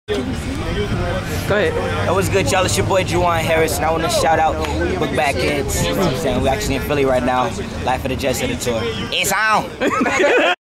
Our boy Jawan Harris gave a shout out to the Book Bag Kidz
Jawan Harris gives the Book Bag Kidz a shout out